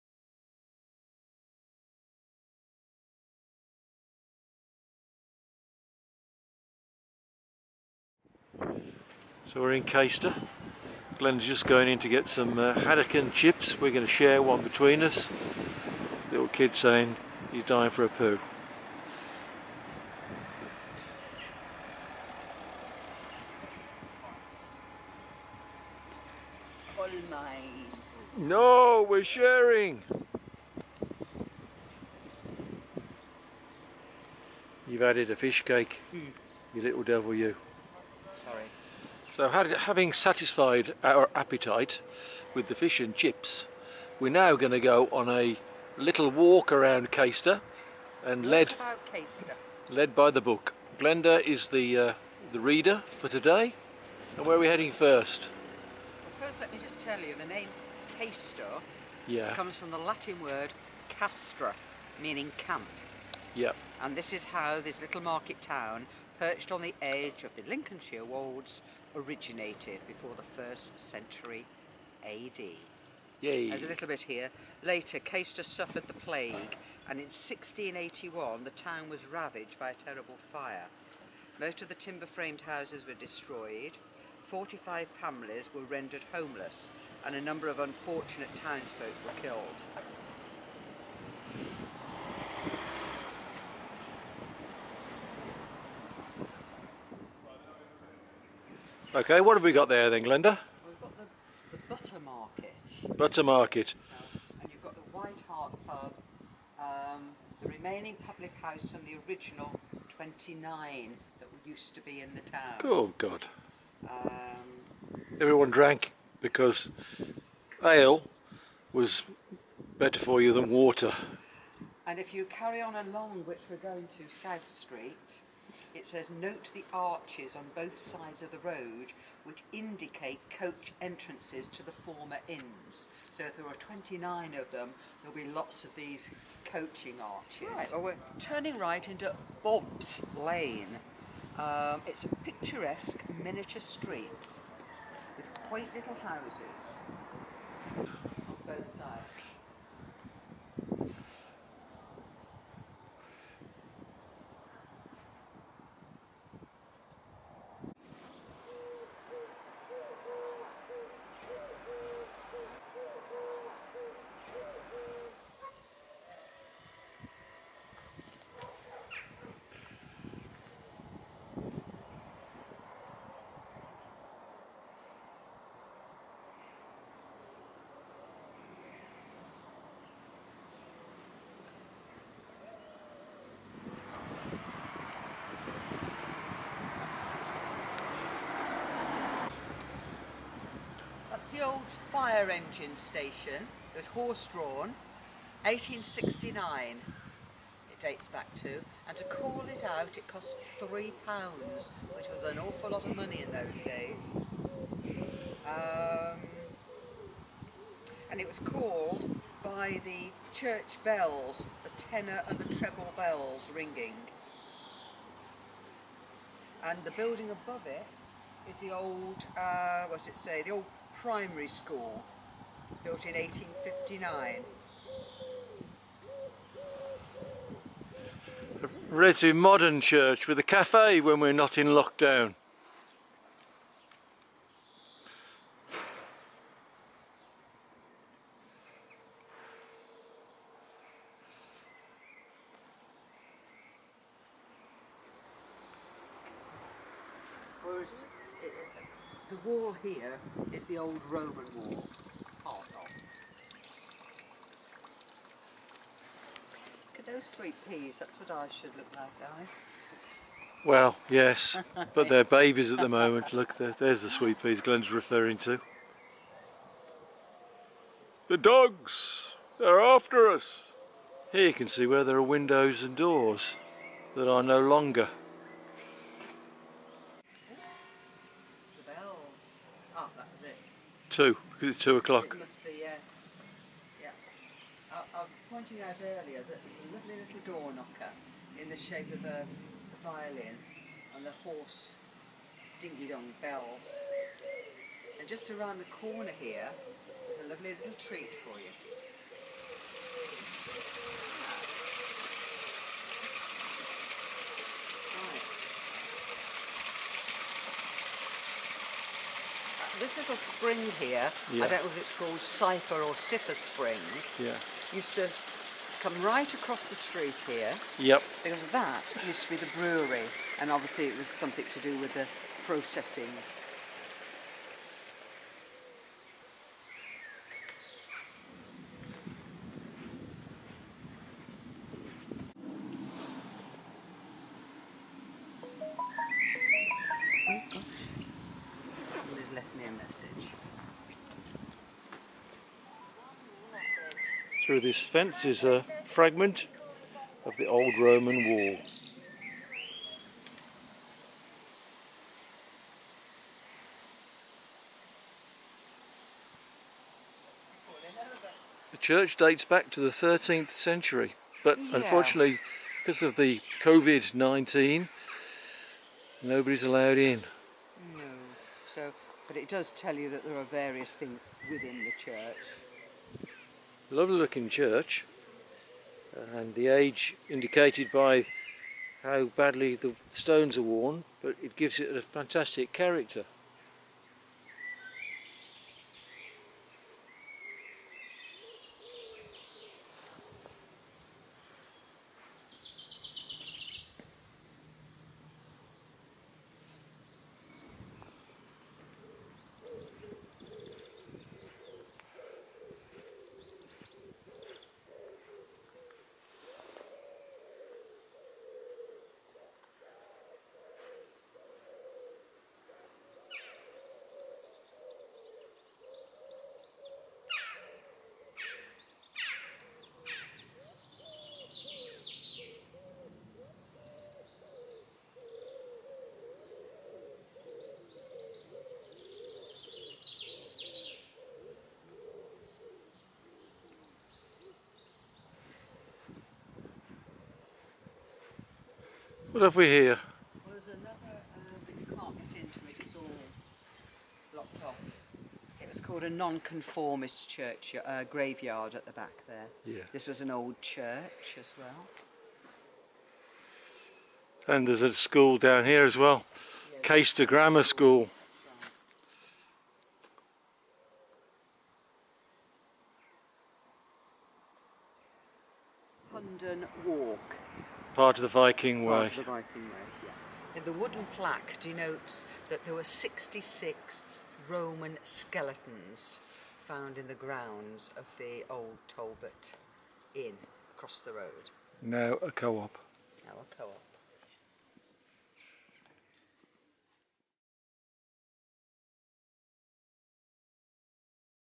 Fish, Chips and walking tour of historic Caistor
I must have visited thousands of different destinations in the past and absorbed little from the experience which is why these day and for a while now Glenda and I make a point of really looking. It is so easy to record events like this one and in doing so it becomes an outlet for creativity both for me and Glenda. I cant say that it helps with the memory at least not in the short term but we do enjoy looking back. Effectively in moments when nothing much is happening in our lives we can replay time. As a destination Caistor was lovely. Not only looking around the town but even the weather was just right.